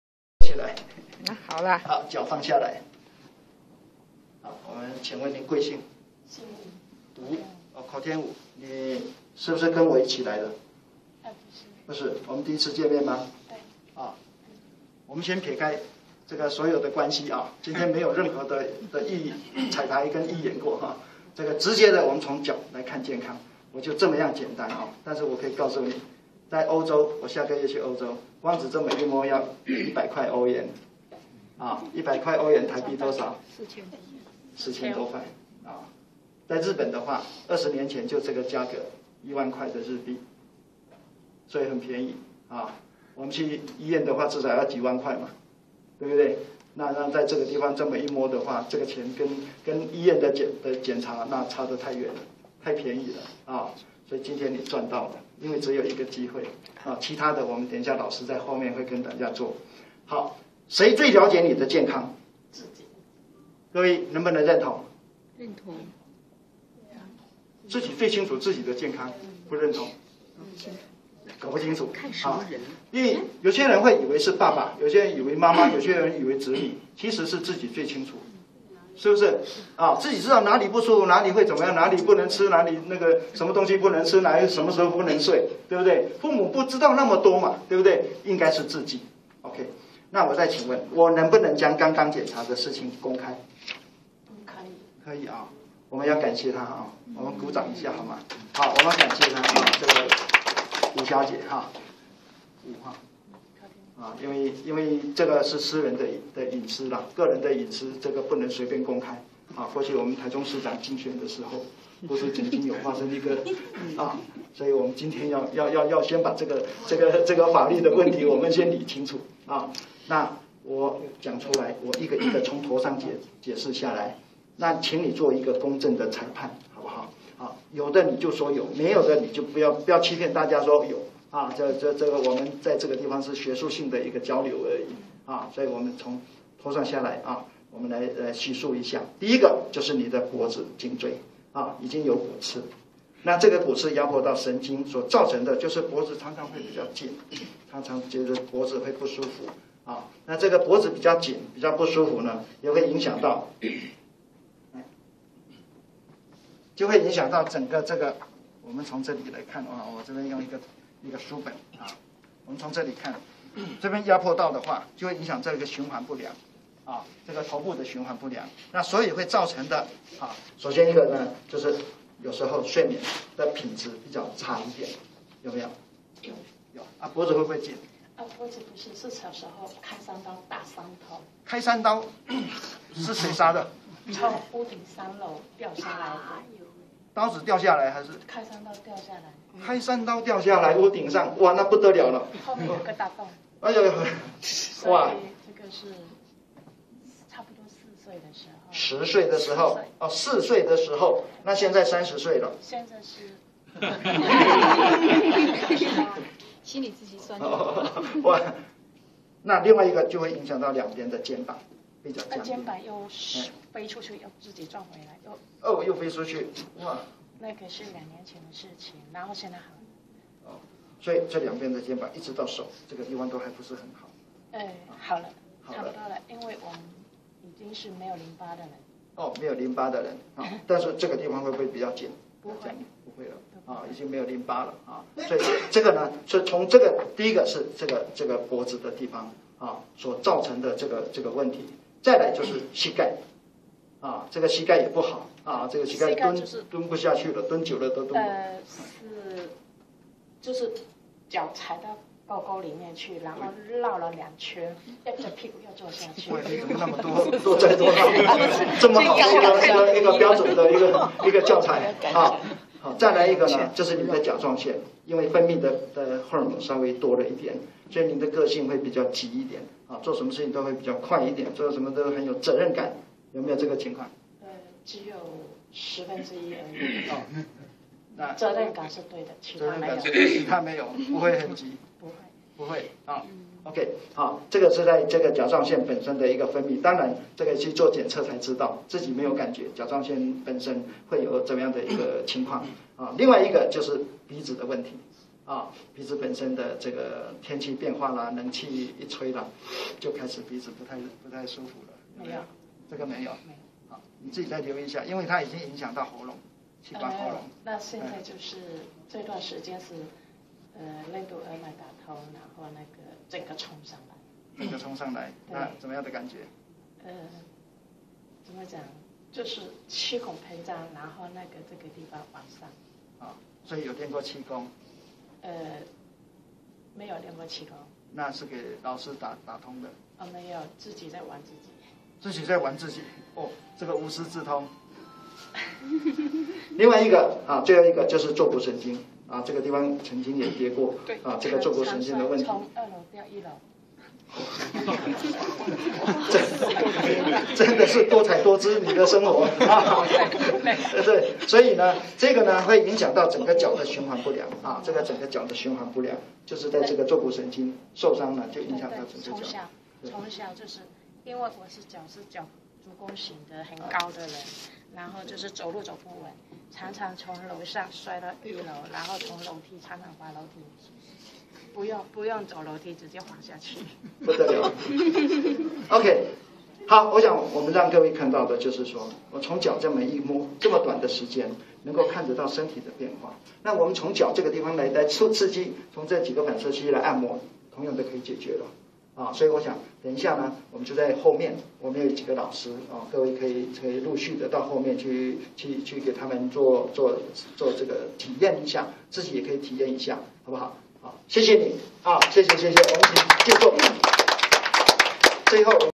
陳茂松創會會長示範無痛診斷VIDEO2338
若石健康法及能量光場的比對現場自願者的當場完整示範